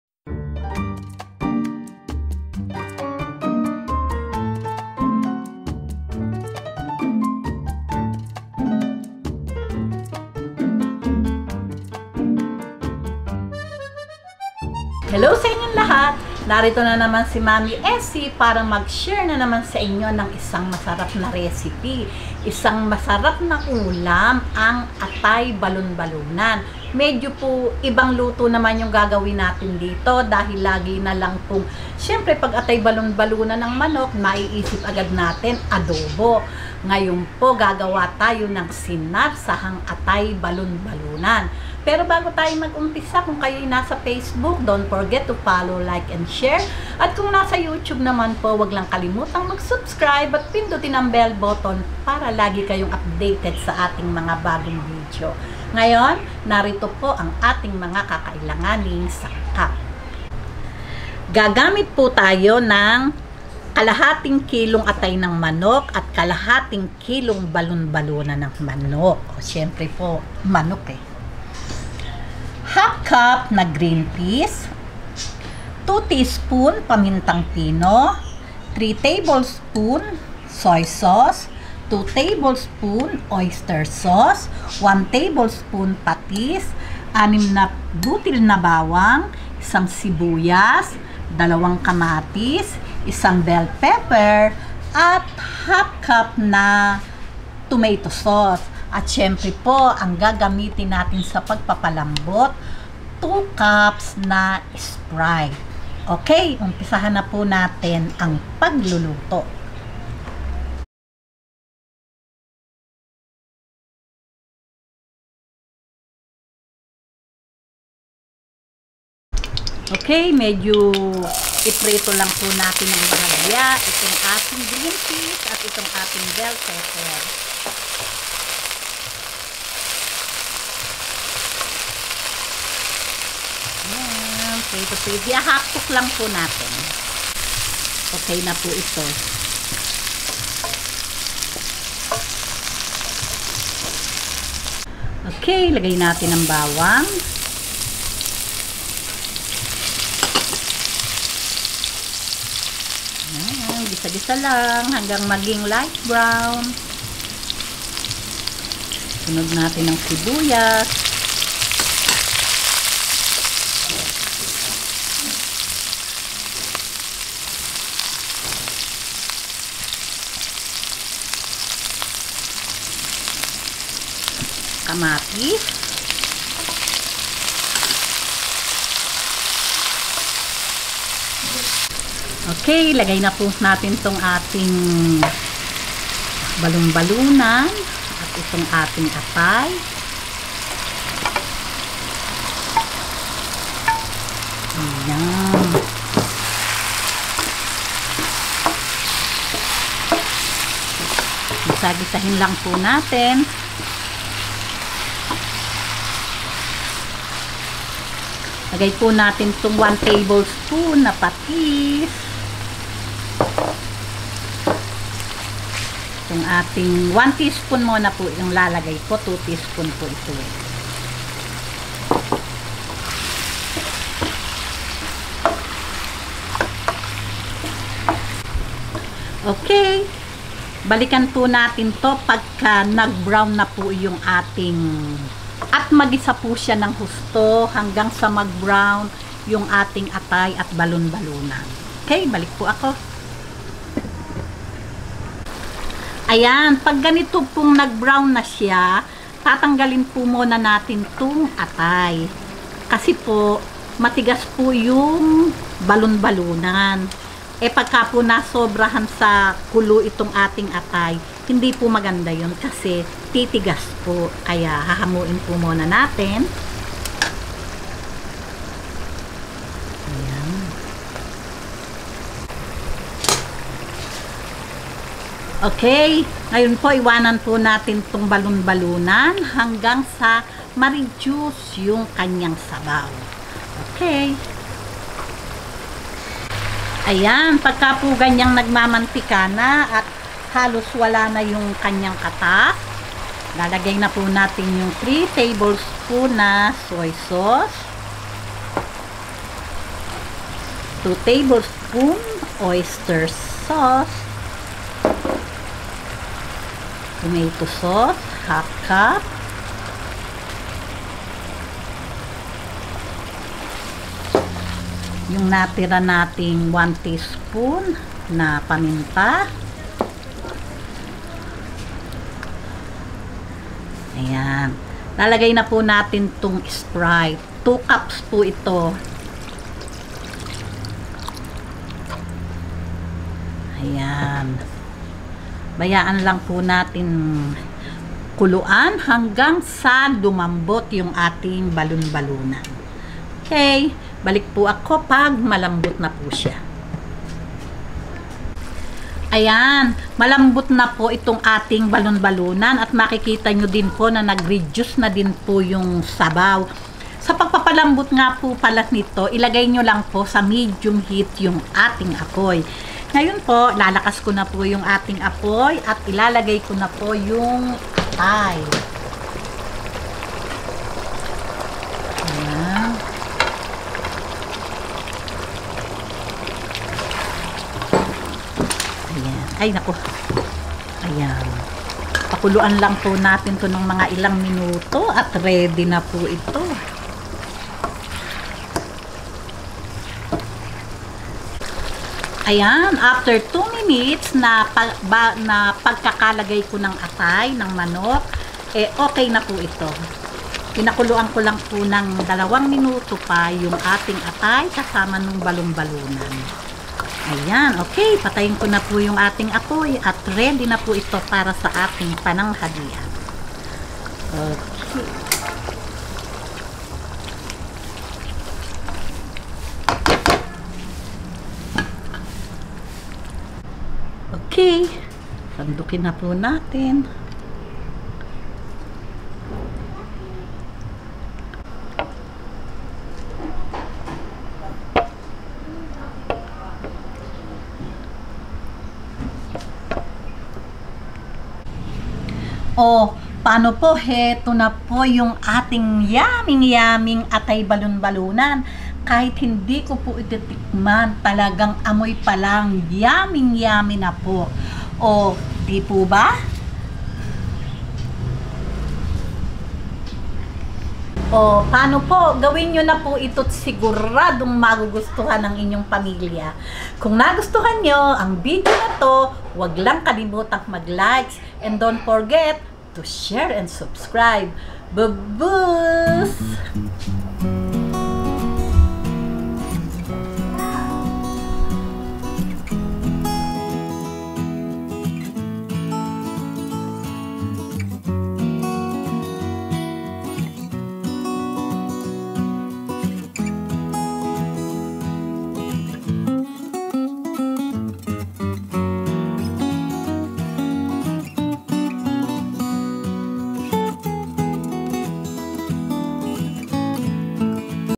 SINARSAHANG ATAY BALUNBALUNAN NG MANOK | KAKAIBANG LUTO SA ATAY AT BALUNBALUNAN NG MANOK
SINARSAHANG ATAY BALUNBALUNAN NG MANOK | KAKAIBANG LUTO SA ATAY AT BALUNBALUNAN NG MANOK | ULAM PINOY | ESIE AUSTRIA

INGREDIENTS:
1/2 kg chicken liver
1/2 kg chicken gizzard/balunbalunan
1/2 cup green peas
2 tsp ground pepper
3 tbsp soy sauce
2 tbsp oyster sauce
1 tbsp patis
6 pcs garlic
1 onion
2 pcs tomatoes
1 bell pepper
1/2 cup tomato sauce
2 cup sprite or any lemon soda